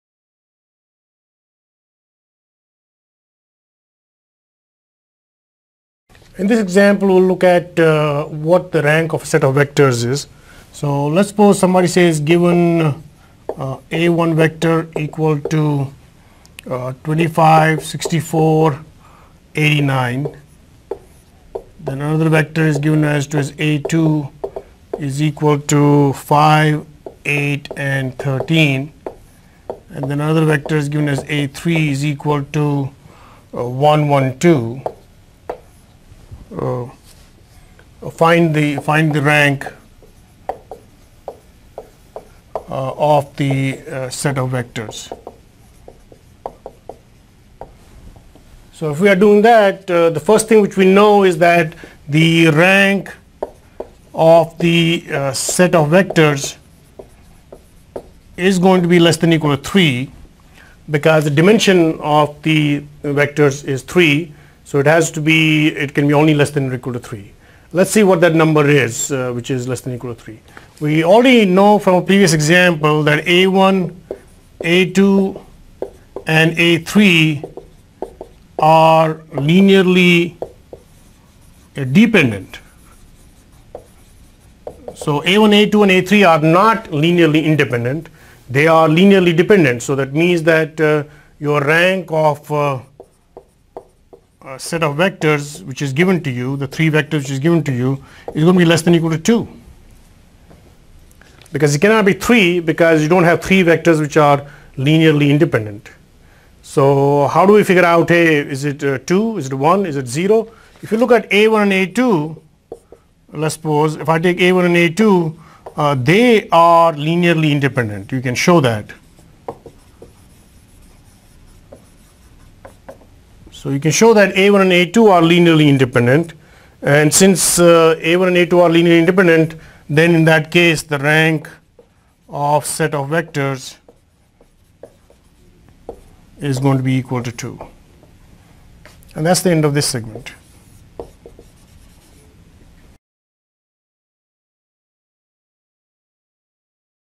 Chapter 04.02: Lesson: Rank of a Set of Vectors: Example 2
Learn via an example rank of a set of vectors. For more videos and resources on this topic, please